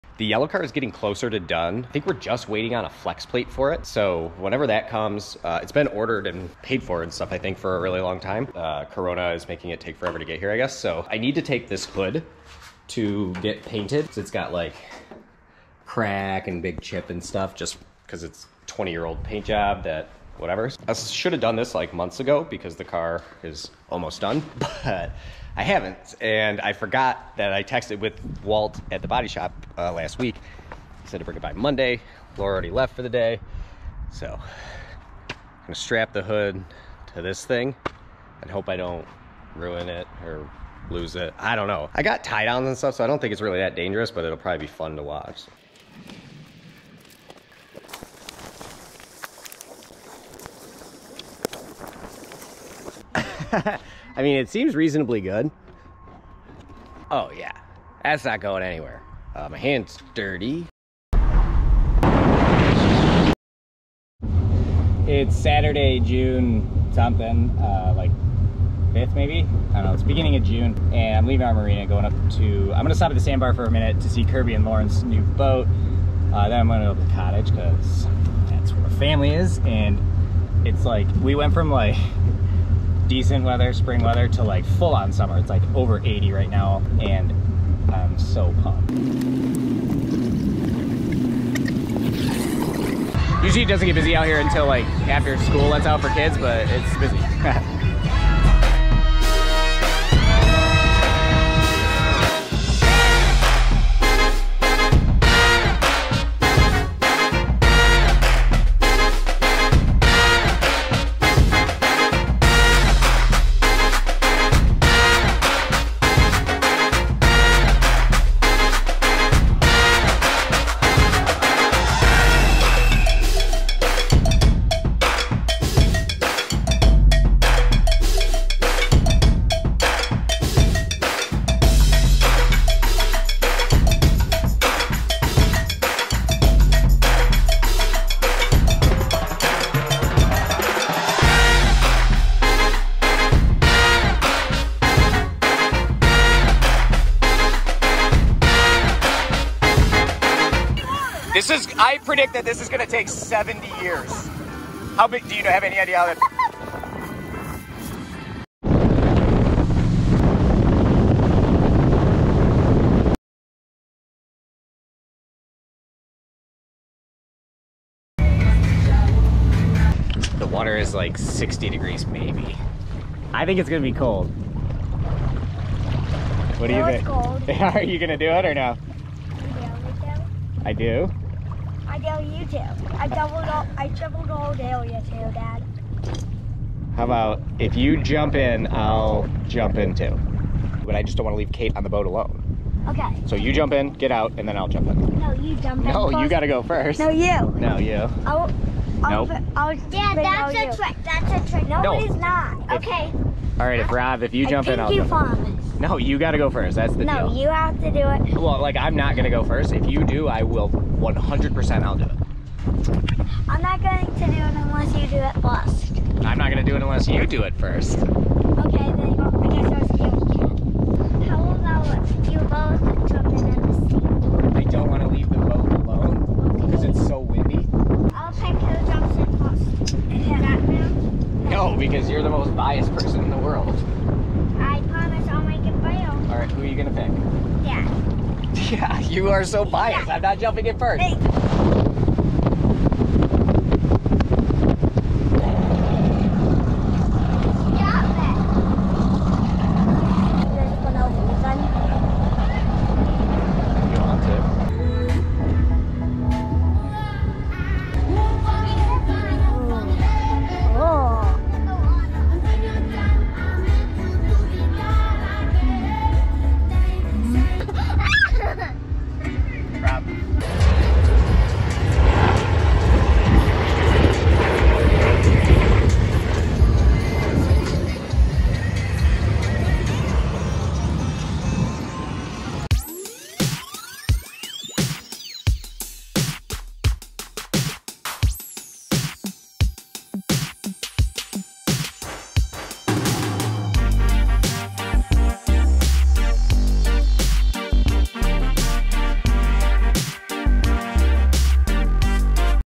Sandbar Saturday - Torch Lake, Michigan June 5th vlog - C43AMG is ultimate cargo vehicle
First minute or so is me being hilarious; I strapped our AMC Hornet’s hood to the roof of my Mercedes C43 AMG and drove it to the bodyshop.

I took the boat up to the North end of Torch Lake last Saturday, but stopped at the sandbar to fly the drone and hang with my buds!

Torch Lake Sunset, Torch Lake Sandbar, Moomba Outback V

DJI Spark Drone, GoPro Hero 9, iPhone 11.  Edited on iPad Pro LumaFusion